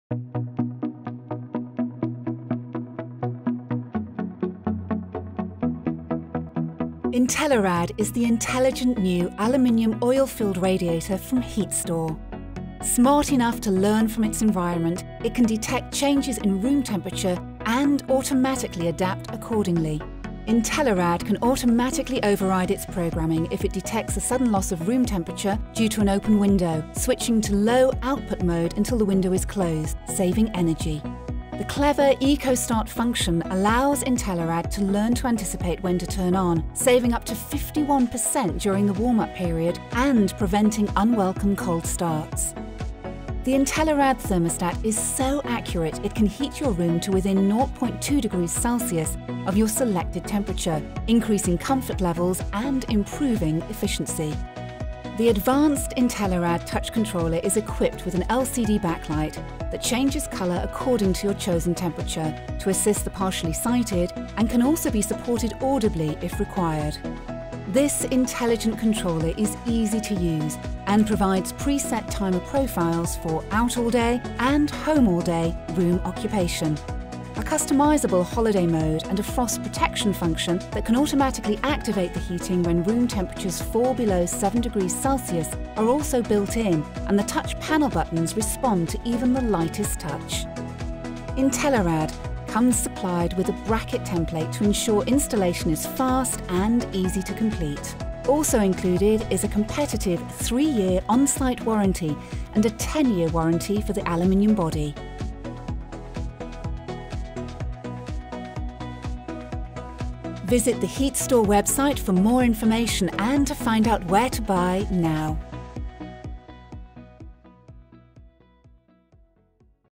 Heatstore Intelirad Radiators - Introduction
Intelirad electric radiators are LOT20 compliant and ideal for reducing heating bills in homes and businesses.

Intelirad uses advanced technology to deliver comfort, control and optimised efficiency, incorporating unique energy-saving features that are not available on similar products.